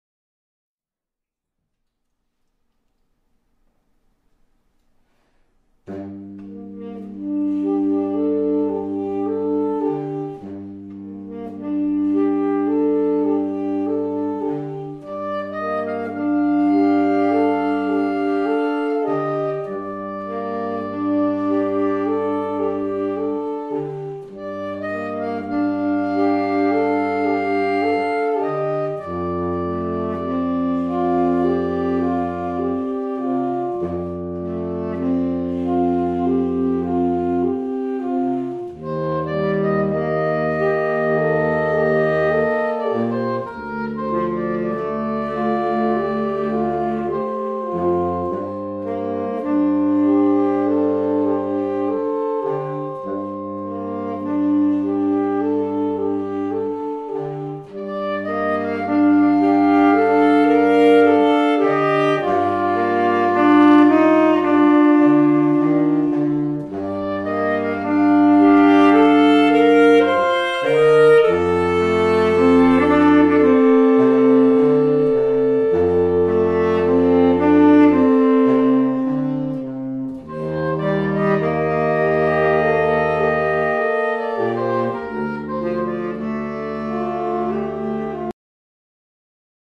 saxophone quartet with lefreQue (love song)
In this video a (amateur) saxophone Quartet will demonstrate the Saxophone with lefreQue.
In the video "Saxophone Quartet without lefreQue (love song)" they will do the same without lefreQue,
so you can hear the difference.